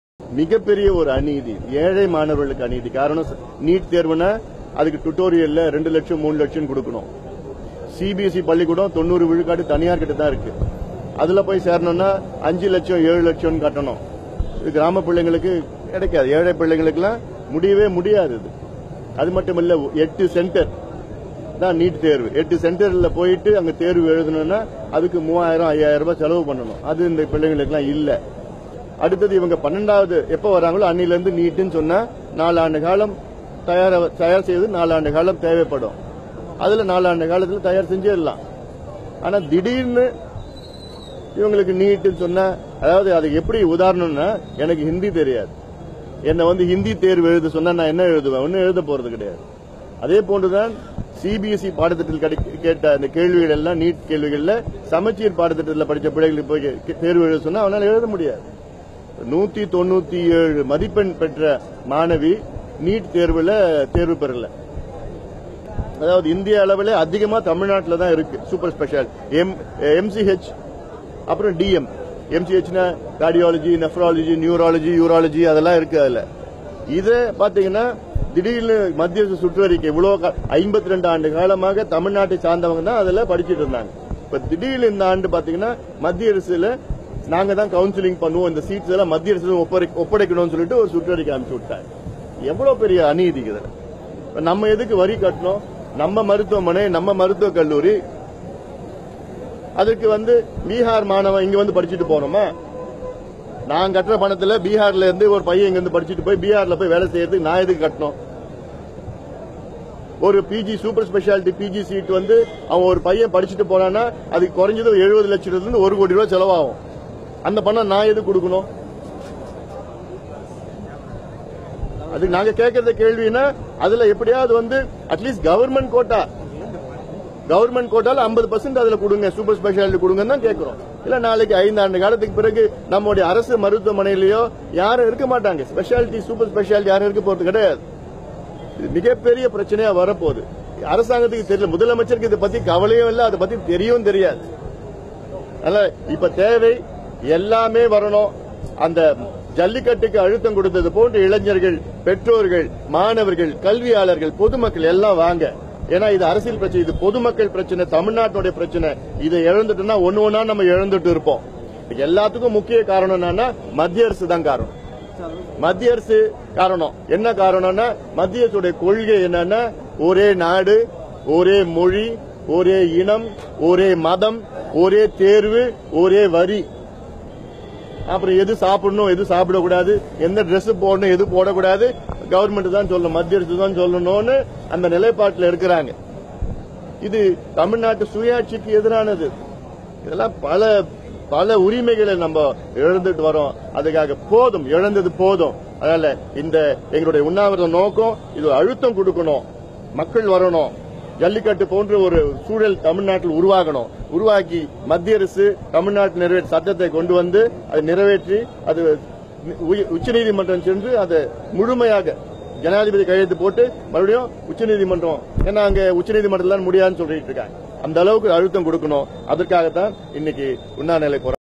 தமிழகத்தில் நான் செலுத்தும் வரி பணத்தில் பீகார் மாணவன் இங்கு மருத்துவம் படித்து விட்டு அங்கு சென்று
தமிழகத்தில் நான் செலுத்தும் வரி பணத்தில் பீகார் மாணவன் இங்கு மருத்துவம் படித்து விட்டு அங்கு சென்று வேலை செய்வது என்ன நியாயம்.